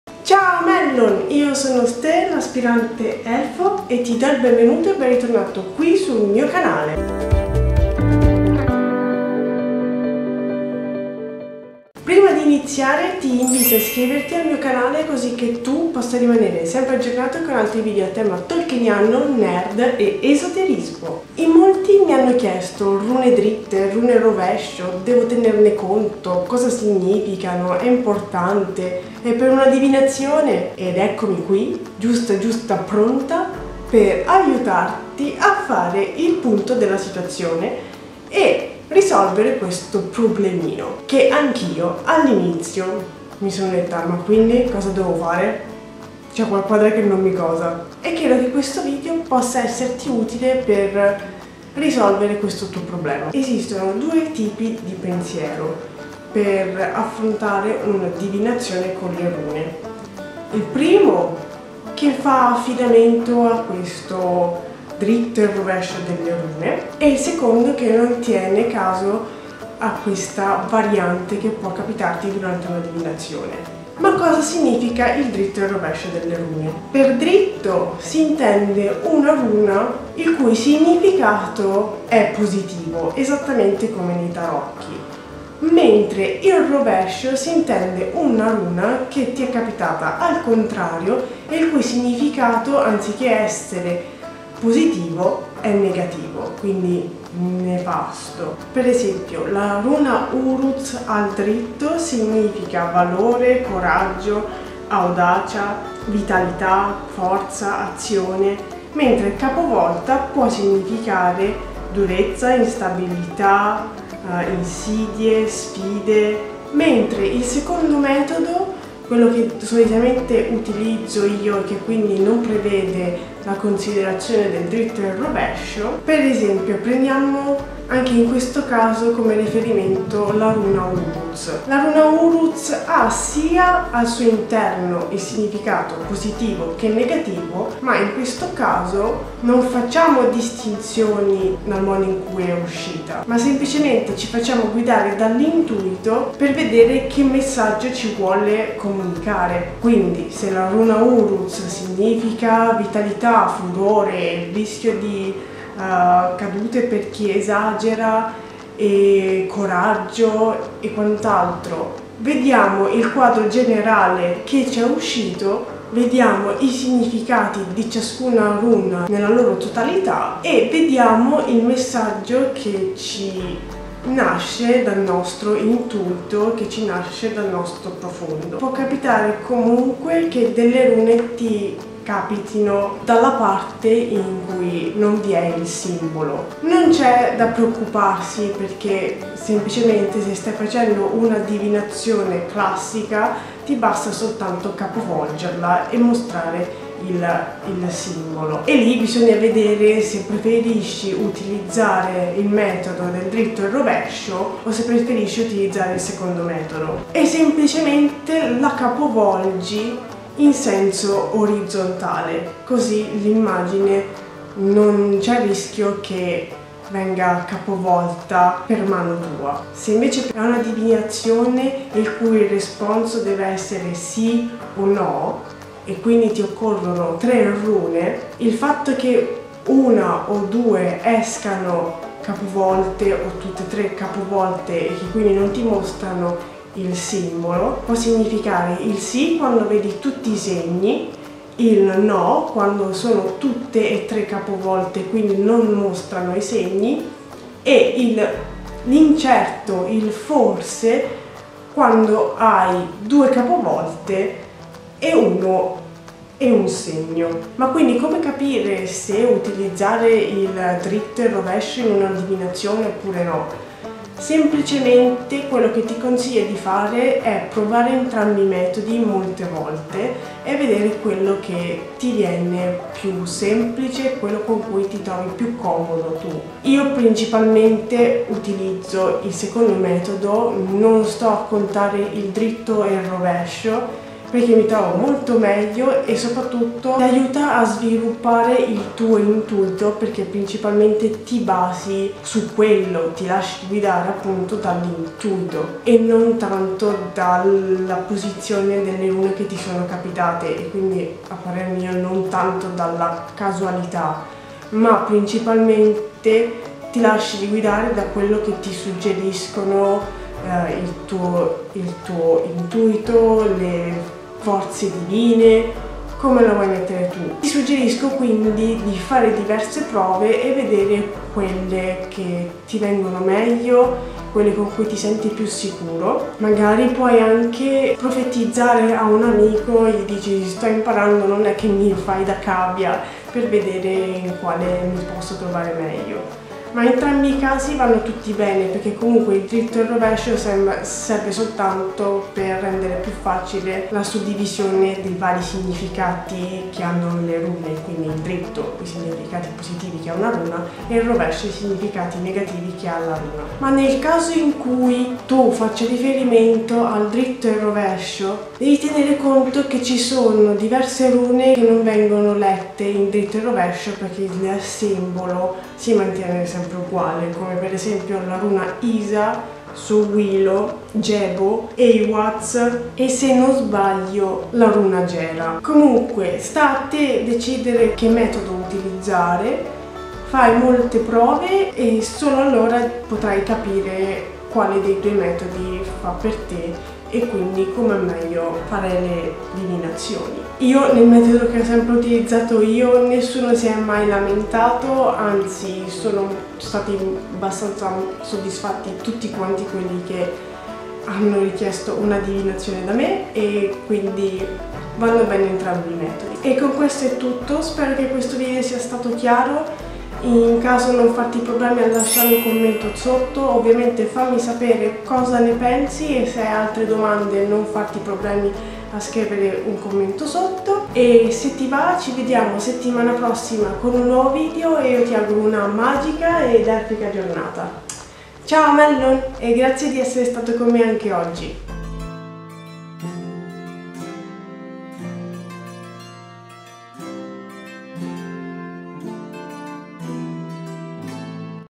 RUNE DRITTE e ROVESCIO: Cosa FARE in una DIVINAZIONE?
In tantissimi Mellyn mi hanno chiesto cosa vuol dire Rune Dritte e Rune Rovescio e cosa bisogna fare al riguardo. Così ho pensato che fosse più utile e facile rispondere a questo dubbio con un video esplicativo e porre rimedio a questo dilemma!

Spero che il video sia chiaro! In caso di altre domande o dubbi non farti problemi a scrivermi!

★Finale: shorturl.at/cdpRV